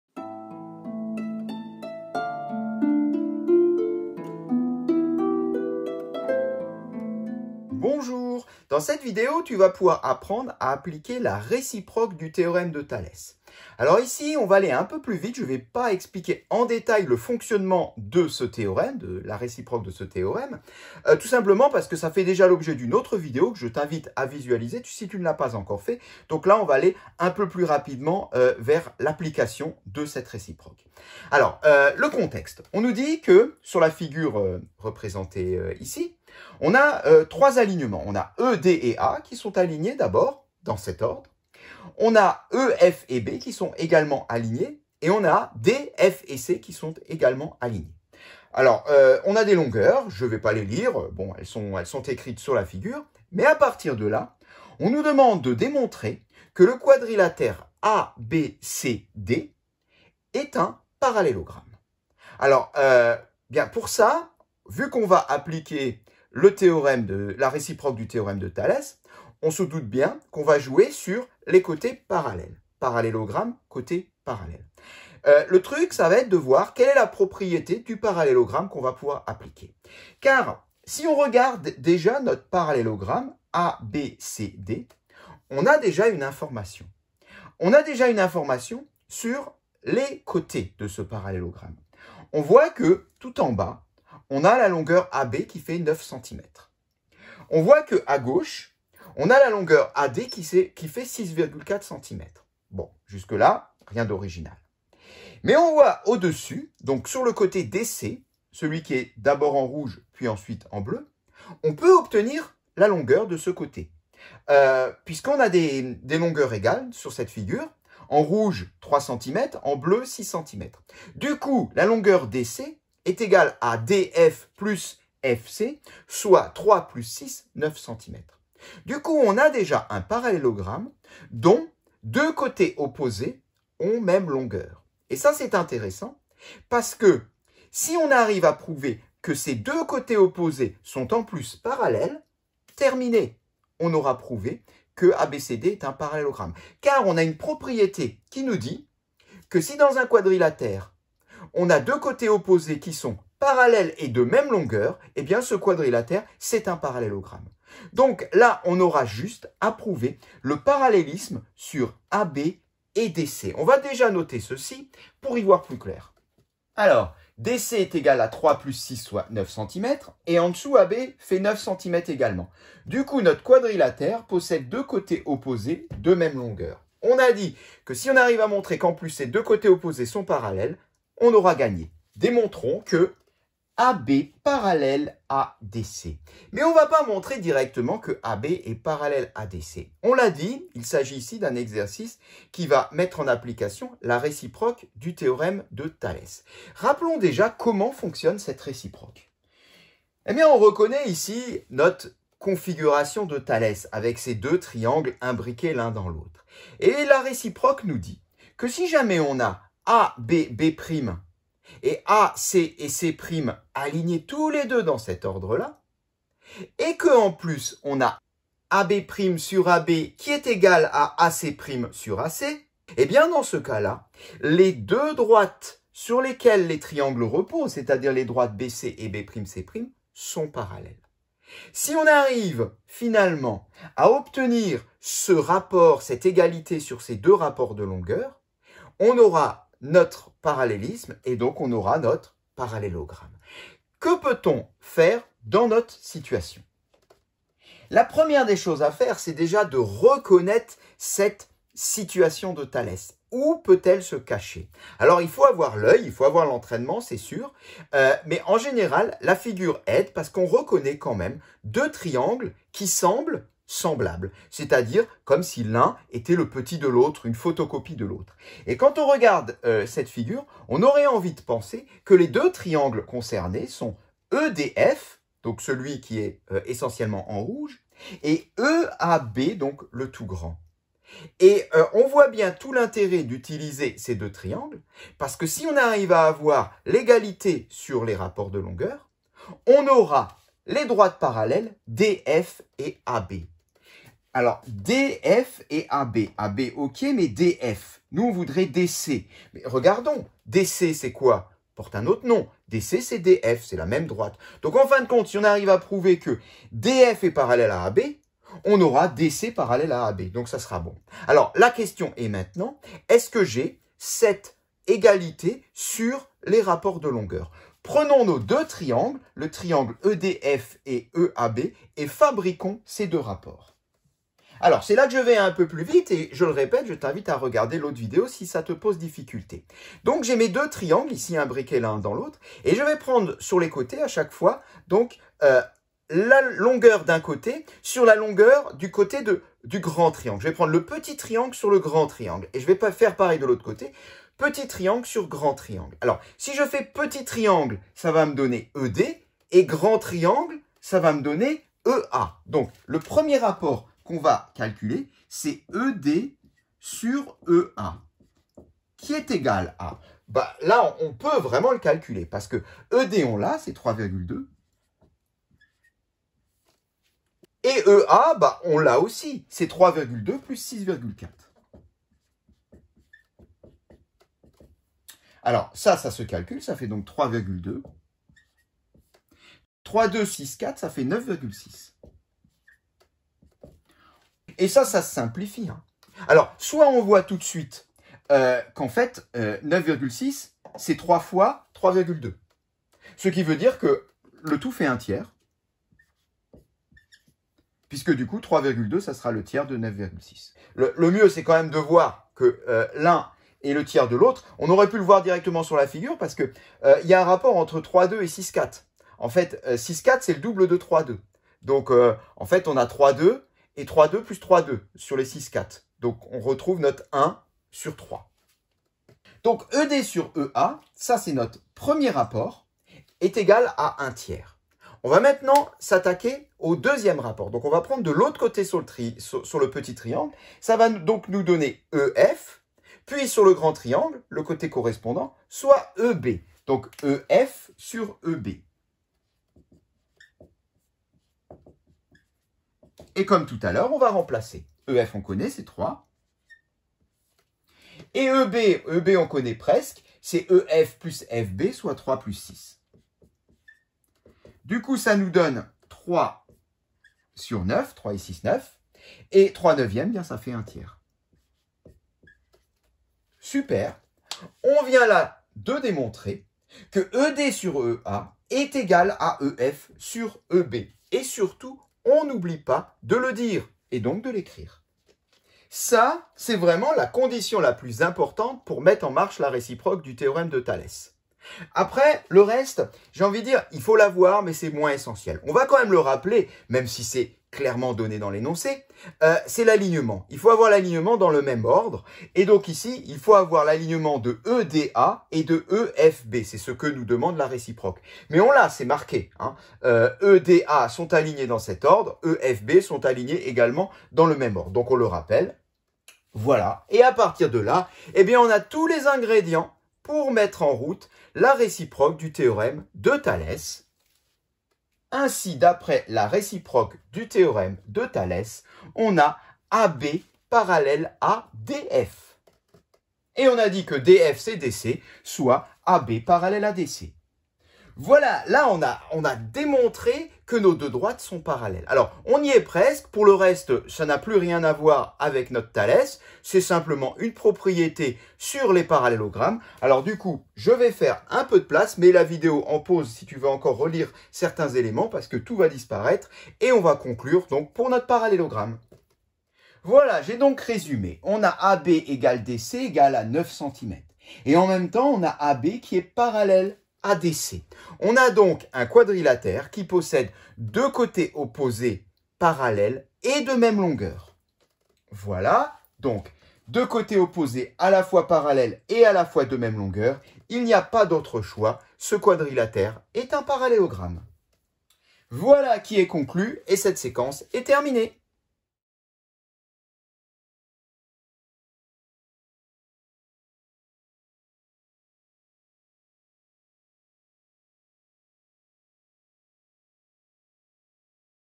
Appliquer la réciproque du théorème de Thalès (2) - Quatrième
Dans cette vidéo, tu pourras apprendre à démontrer que deux droites sont parallèles en appliquant la réciproque du théorème de Thalès. 👍